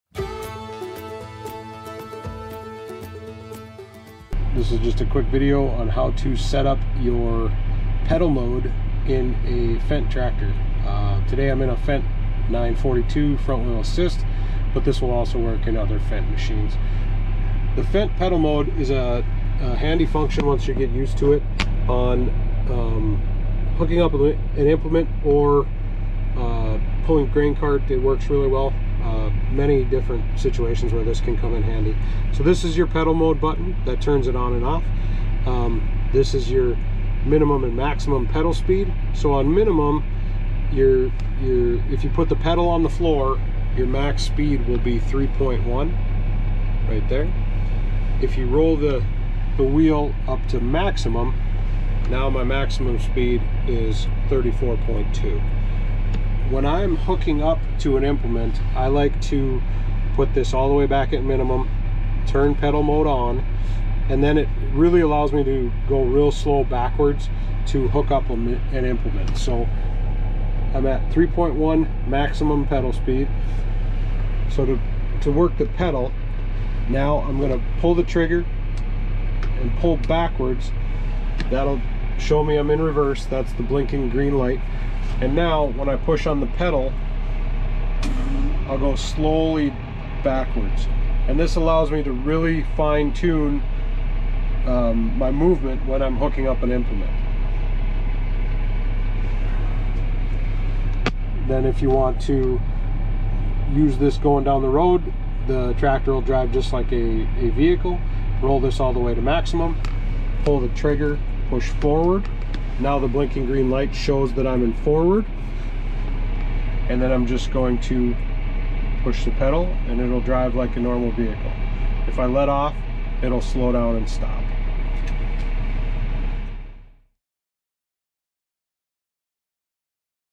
Setting Up Pedal Mode on the Fendt VarioTerminal NT01 Monitor
Watch Nate from Butler demonstrate how to set up your pedal mode on the Fendt VarioTerminal NT01 screen. These instructions apply to the NT01 screens in Fendt 800 Series, Fendt 900 Series, Fendt 1000 Series, Fendt 900MT Series and Fendt 1100MT Series tractors.